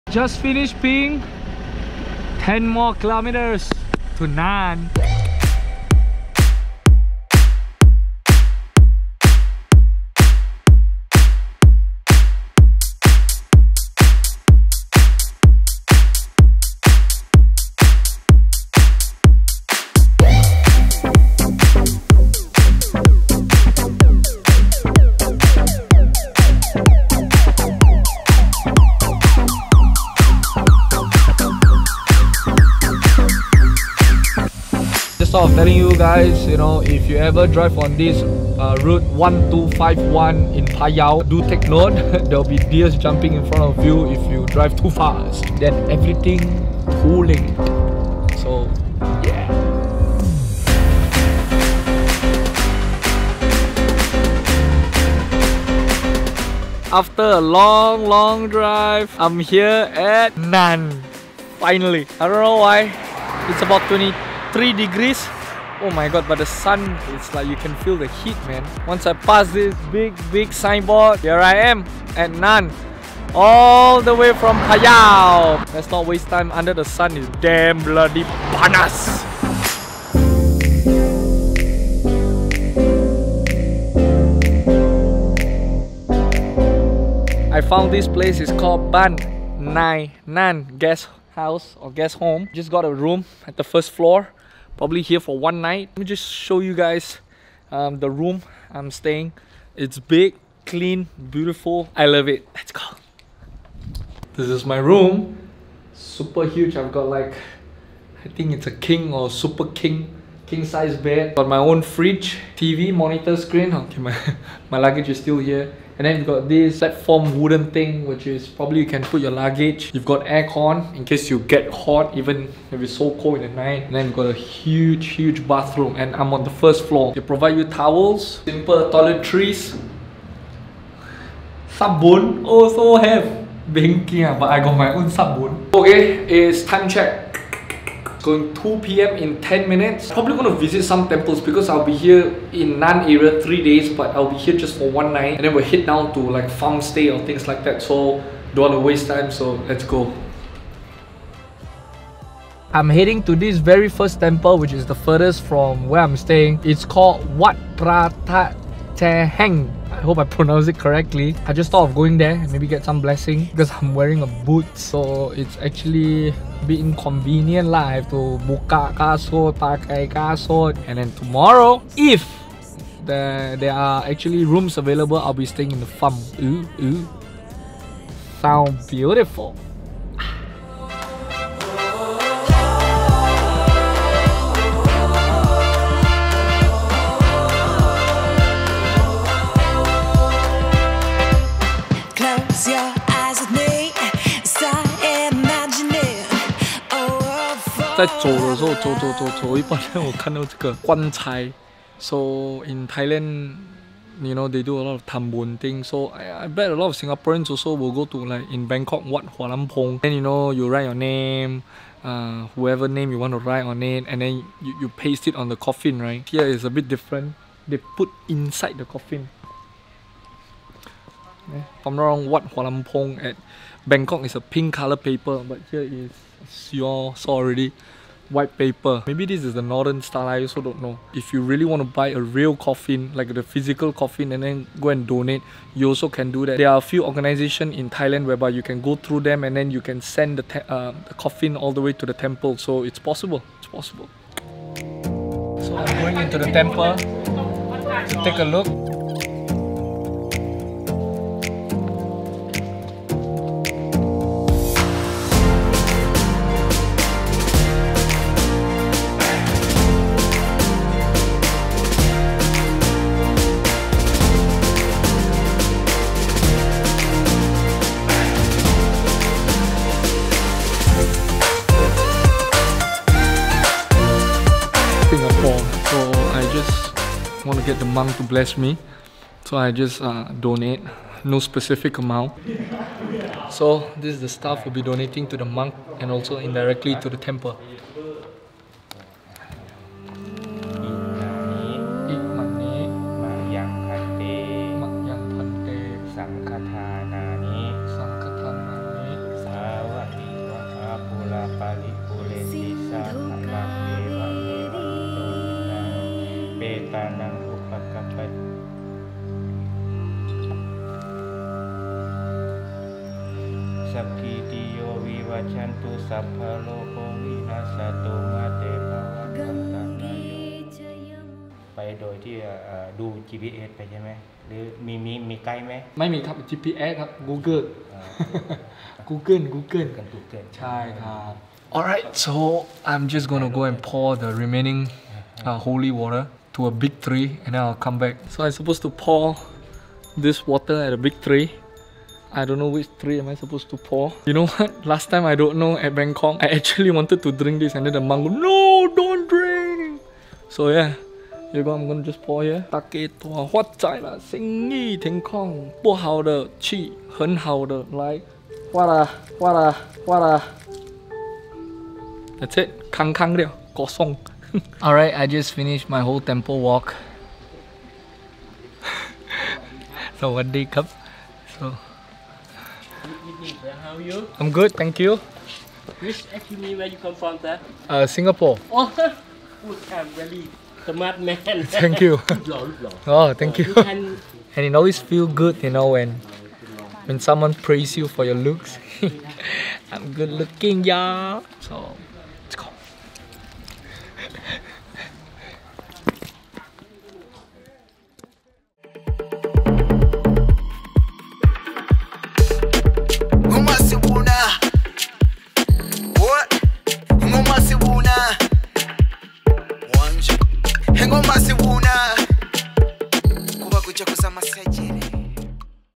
Live Size Coffin In The Temple! - EP11 (Eng & Thai sub)
Wat Phra That Chae haeng was built in the 14th century. Located 200 meters South of Route 1168, East town of Nan province.

The temple derives its name from the Phra That Chae Haeng, the gilded main chedi that contains a relic of the Buddha. Wat Phra That Chae Haeng was founded in 1355. The structures of the temple complex show a mix of several architectural styles, including Lanna and Thai Lü.

Location and Address:

89 มหาพรหม Pha Sing, Mueang Nan District, Nan 55000, Thailand

So enjoy! =)

Gears:
- Canon
- Rode Video Mic Pro +
- Samsung S9+